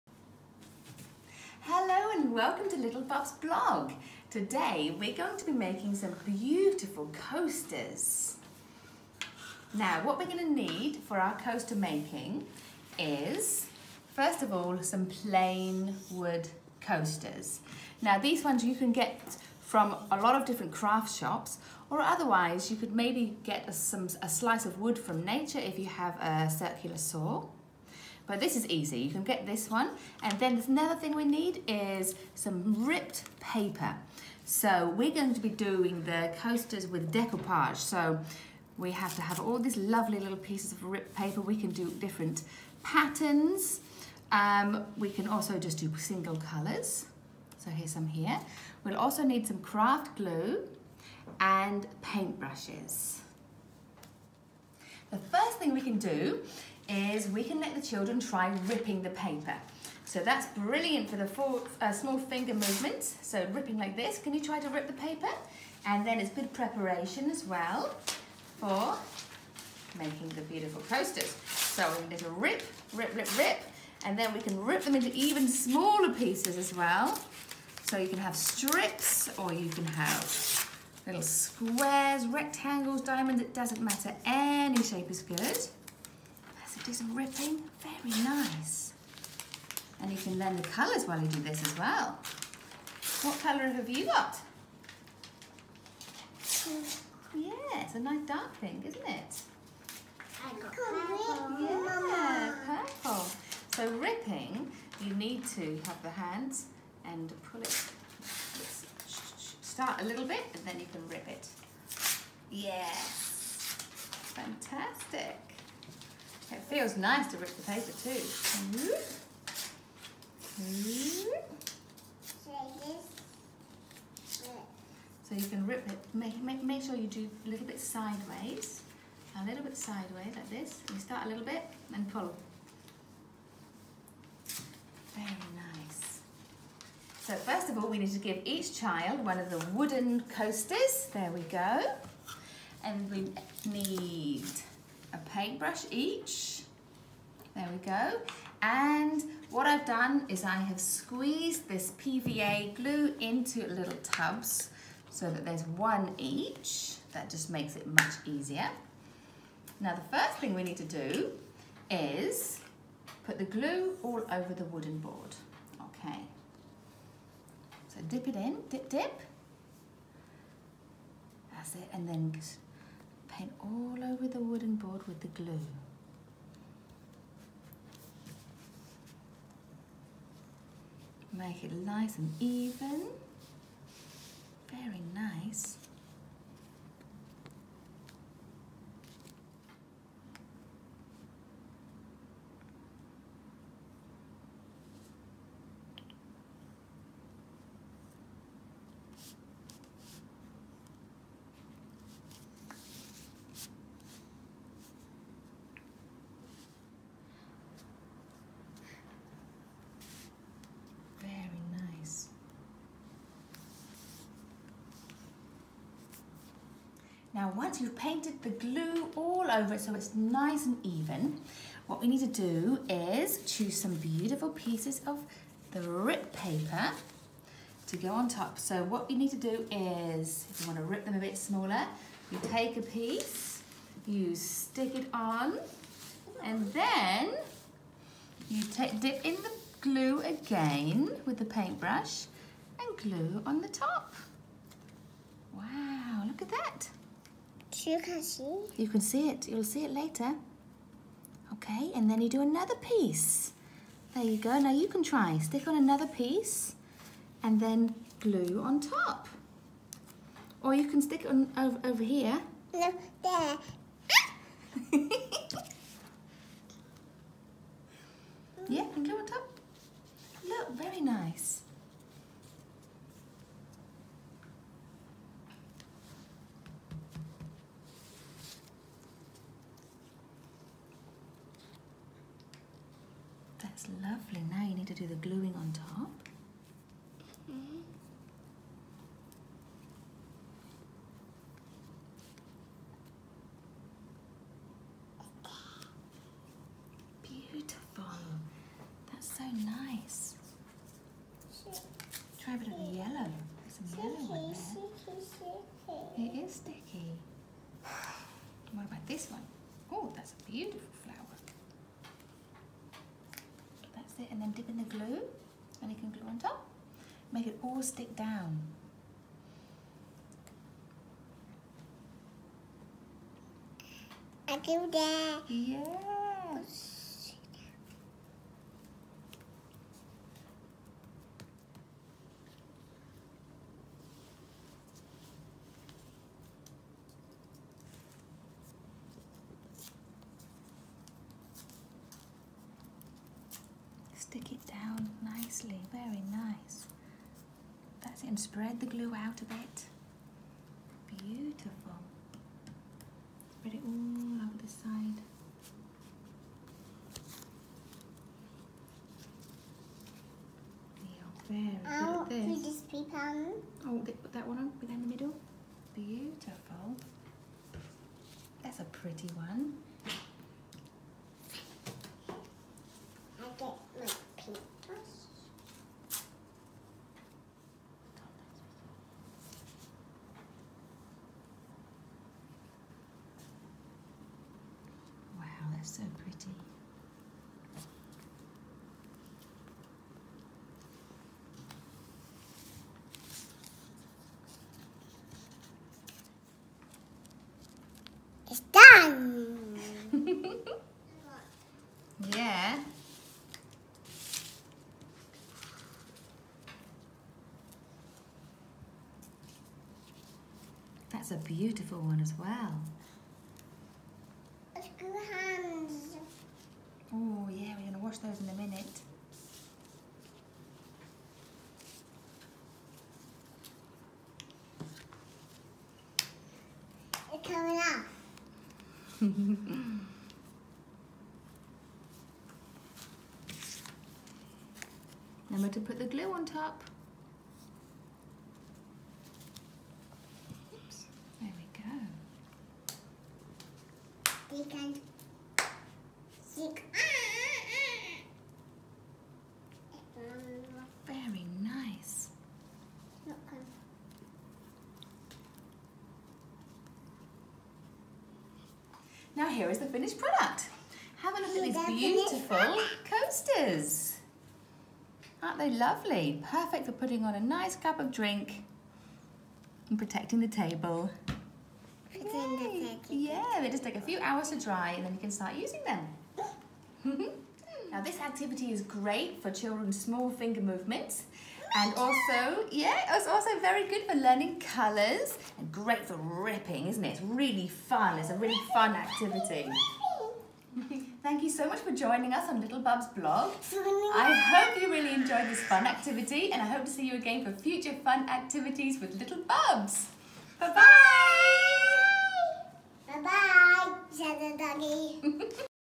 Little Bubs Blog making decoupage coasters
Little Bubs Blog teaching how to make beautiful decoupage coasters using ripped paper and wood glue